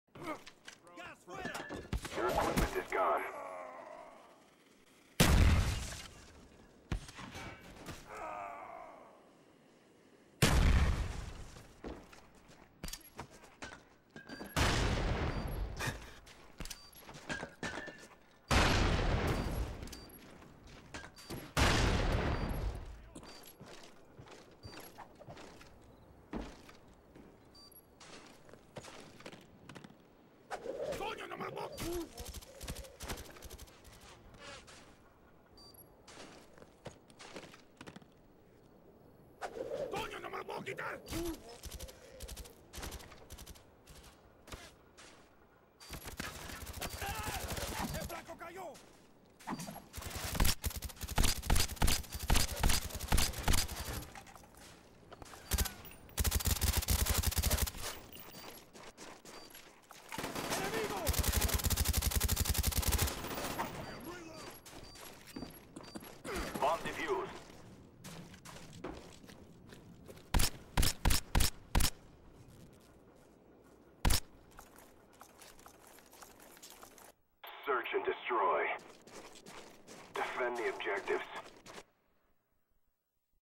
A WTF Moment
it was all a big lag but really funny my friend said she still heard me shooting when they were switching rounds haha